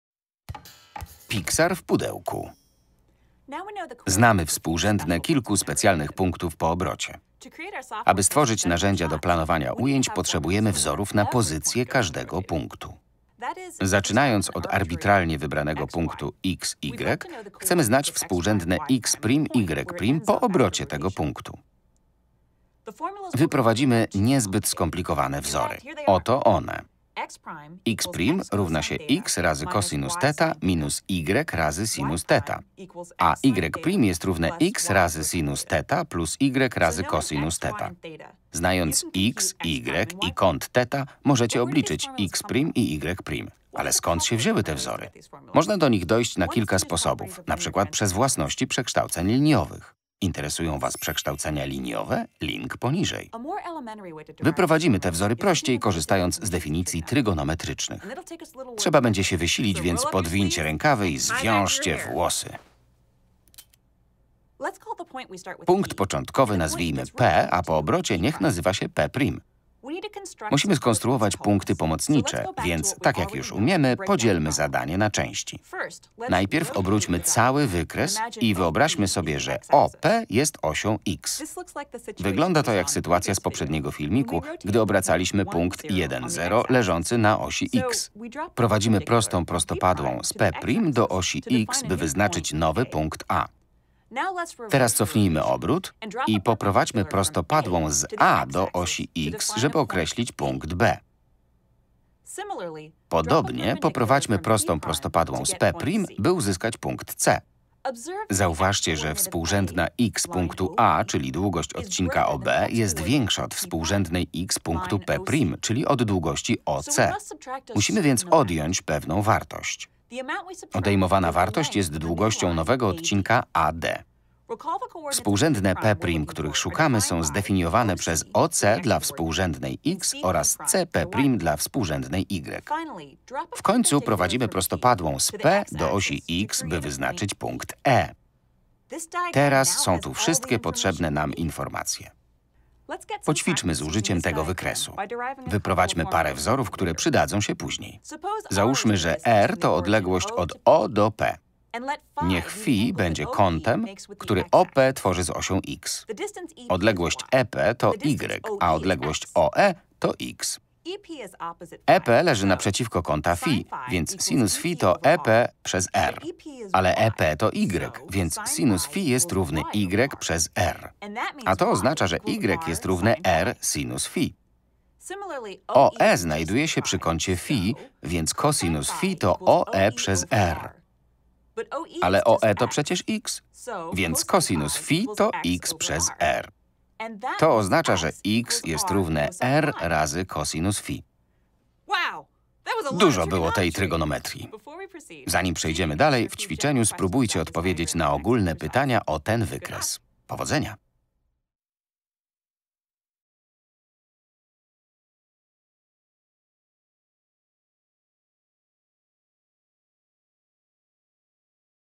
Pixar w pudełku - plany filmowe - matematyka obrotów - 2. Geometria obrotów
Odkryj, razem z Pixarem i Khan Academy, jak wirtualne plany filmowe są tworzone za pomocą przekształceń geometrycznych